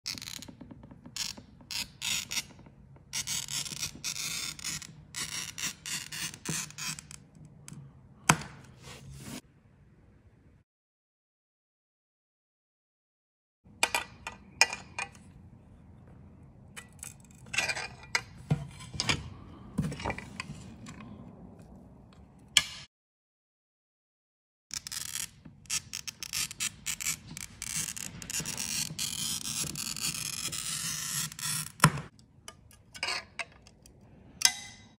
Cut Iridized Glass stainedglass Iridizedglass glass cutting
Cut Iridized glass using this glass cutting technique for stained glass.